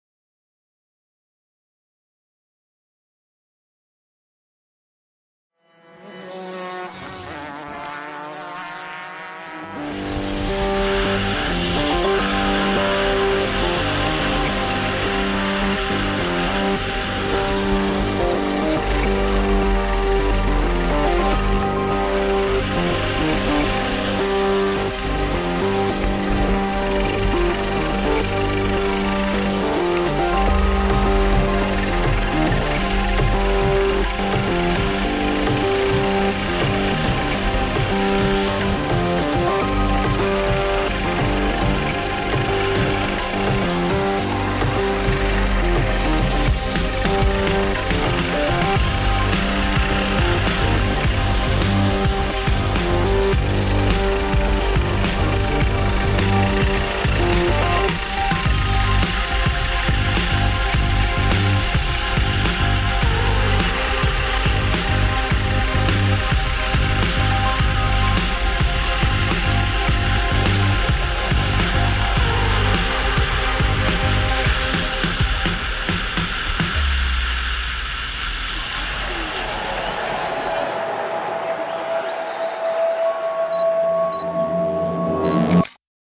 Onboard video in go-kart of the Pista Azzurra in Lido di Jesolo
Discover the track of the Pista Azzurra in Lido di Jesolo with an onboard video of Stefano Pallavicini!

Registration File + Regulations of the 24h Jesolo E.K.T. which will be held on September 16th & 17th 2017

Endurance Kart Trophy by RD Events